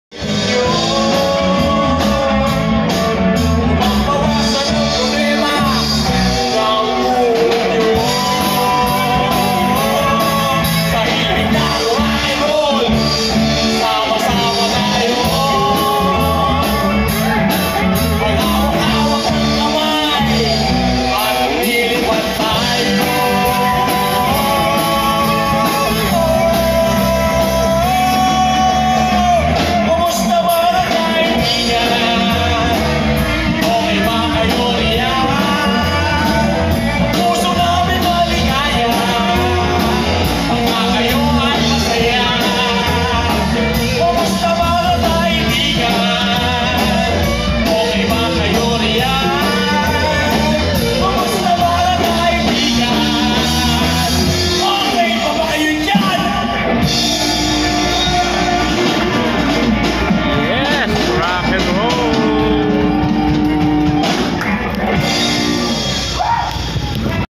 Sa Himig ng Rakenrol
Napadaan lang sa isang Birthday Celebration, Live band music